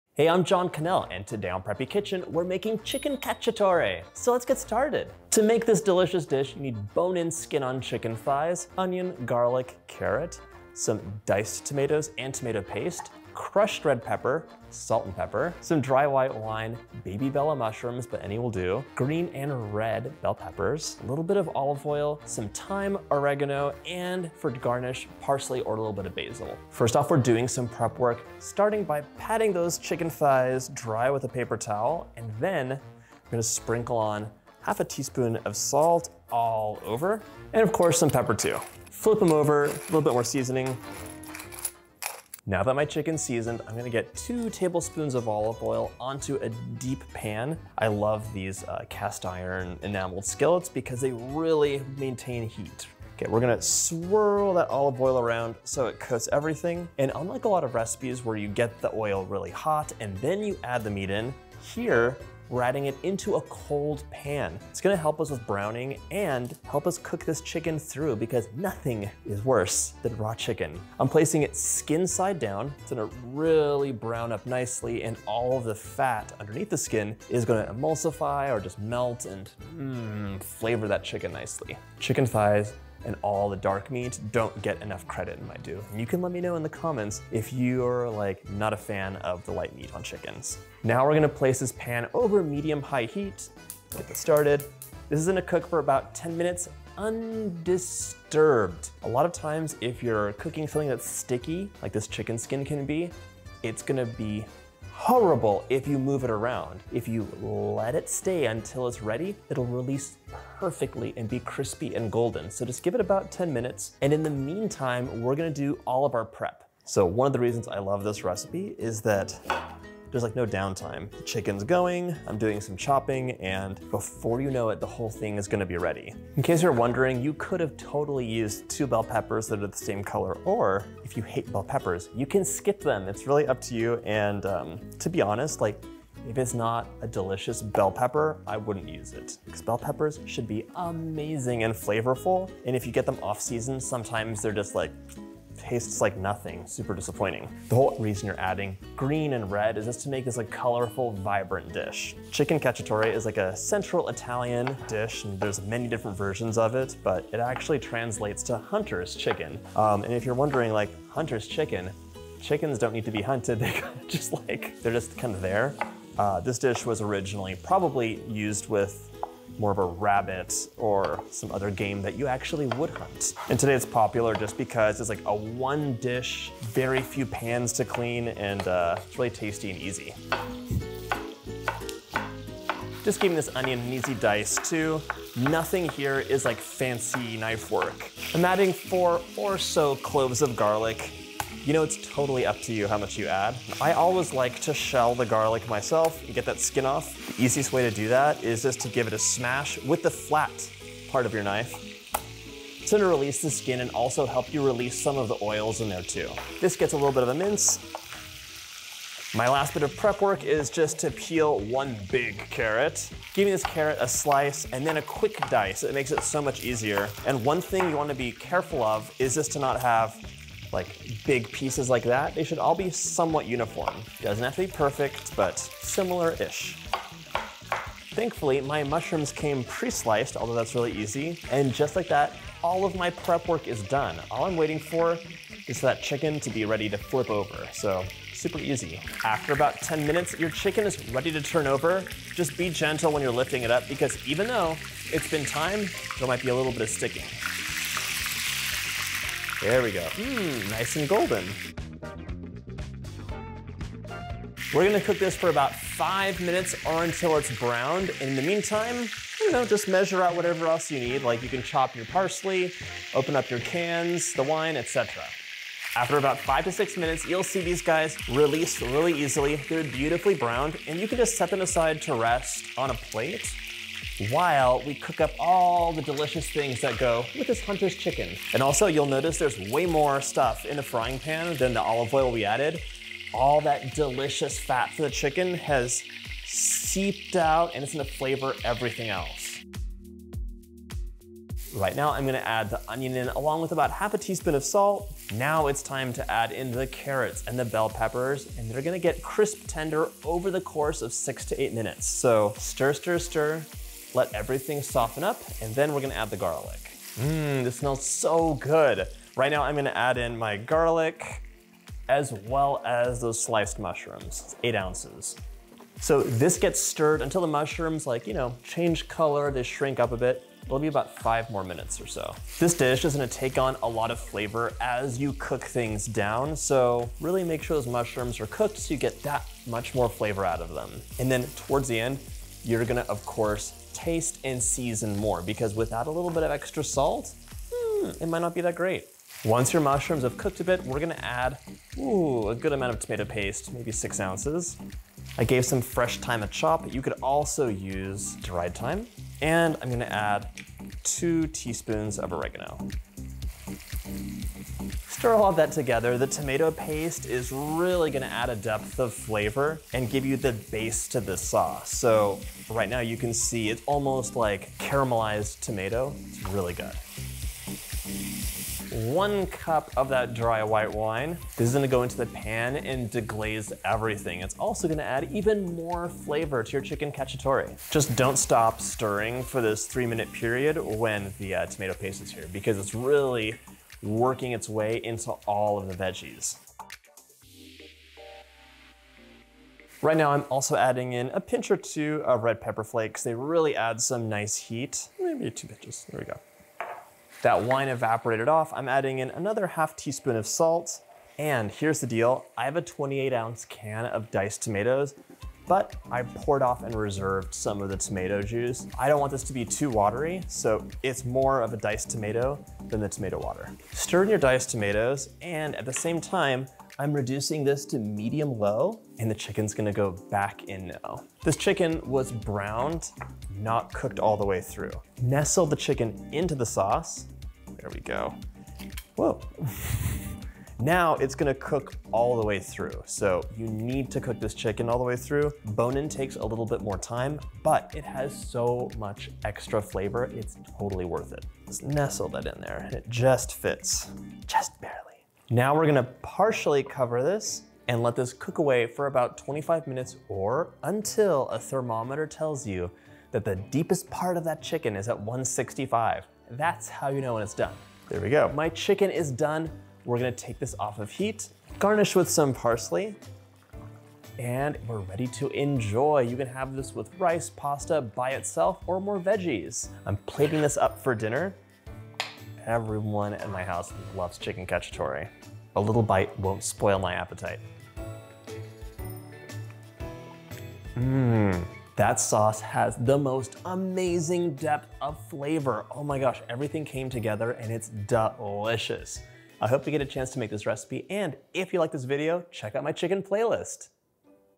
Chicken Cacciatori Recipe | Easy Dinner Recipe
A classic Italian dinner, this Chicken Cacciatore is an easy one-skillet meal that comes together in a flash. Crispy and juicy chicken thighs nestled in a tasty tomato-based sauce, this is an easy chicken recipe that the whole family will love.  This chicken cacciatore is packed with flavor while only using simple everyday ingredients, and you're doing everything in one pan so there's less cleanup!

PRE-ORDER MY BOOK!